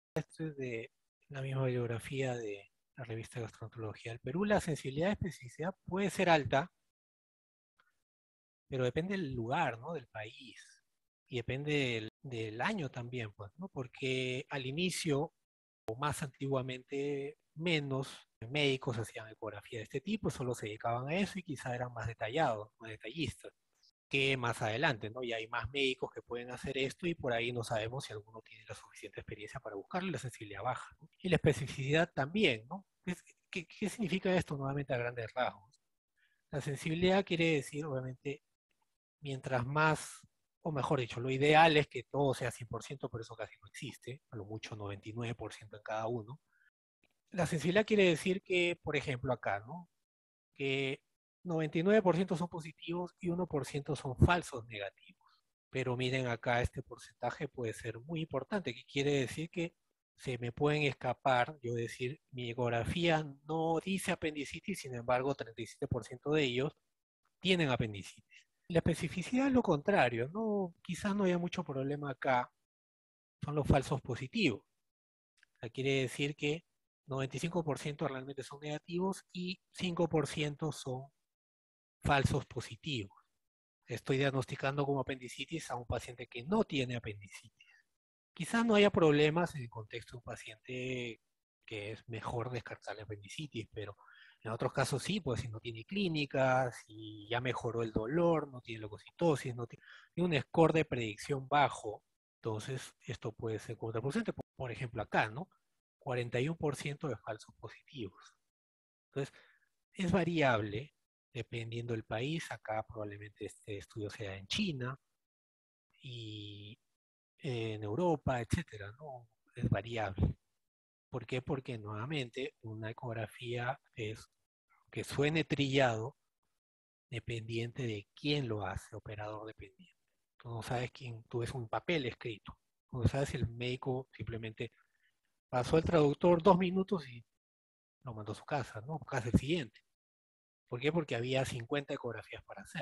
Apendicitis 10: Sensibilidad y especificidad de la ecografía
Bibliografía.
Velásquez Hawkins C, et al. Valor del Ultrasonido en el Diagnóstico y Manejo de la Apendicitis Aguda. Rev. gastroenterol. Perú [Internet]. 2007 Jul [citado 2022 Mar 25] ; 27(3): 259-263.